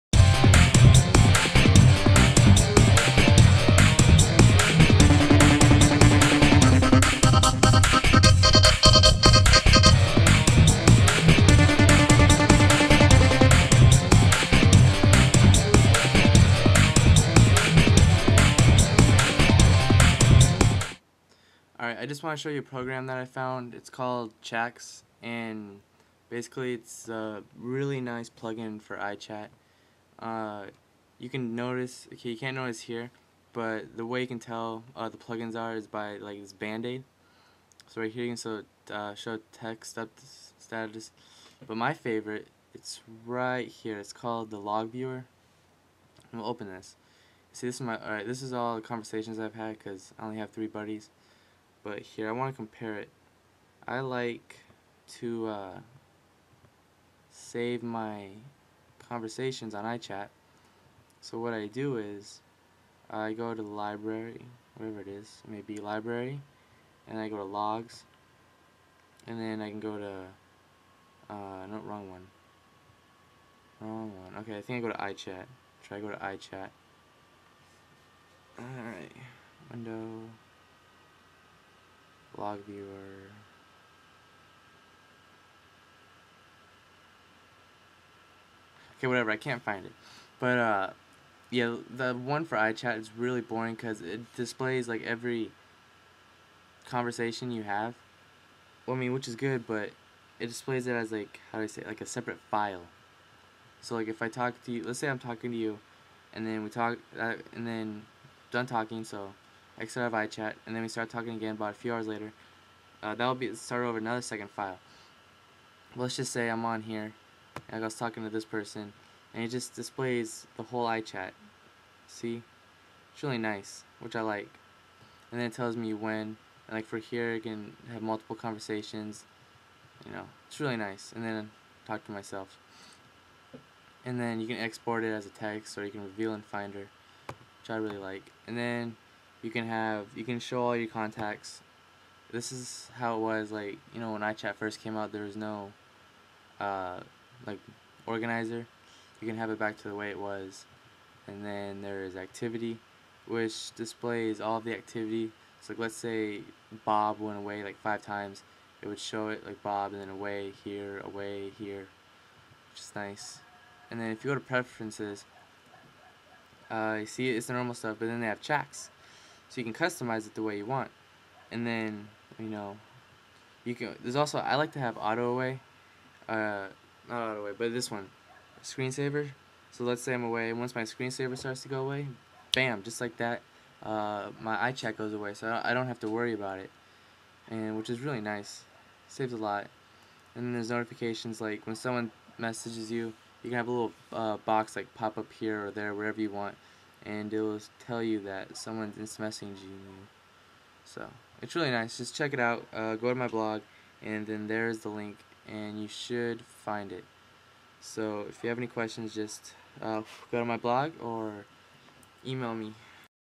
Must Have iChat Plug-In: Chax
- Travis shows you a great plug-in for iChat, Chax